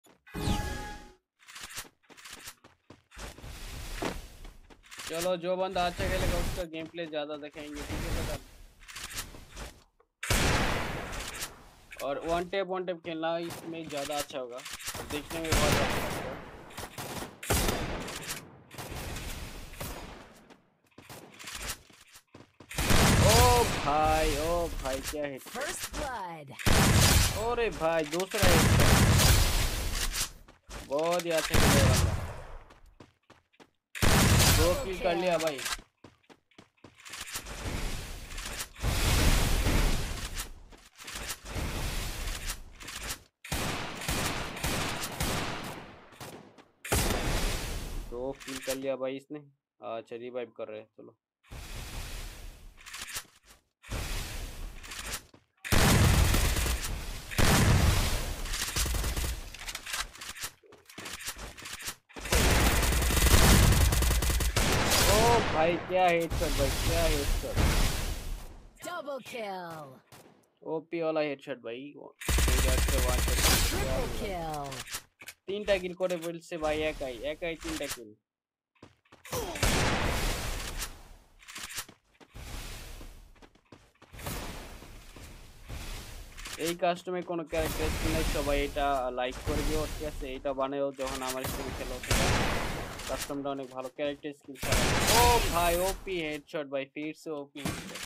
PRO SUBSCRIBER ONE TAP GAMEPLAY (SHAKIL GAMING SG)
🎮 Welcome to the adrenaline-pumping world of Pro Subscriber One Tap Gameplay with Shakil Gaming SG! 🚀 Brace yourselves for an immersive gaming experience like never before. 🔥 In this video, Shakil Gaming SG takes you on a journey through intense one-tap gameplay that separates the pros from the rest. 🏆 Witness unparalleled skills, strategic moves, and jaw-dropping moments that will keep you on the edge of your seat. 🚨 Pro tips, epic wins, and endless entertainment await. 🕹️ Get ready to level up your gaming experience with Shakil Gaming SG! 🌟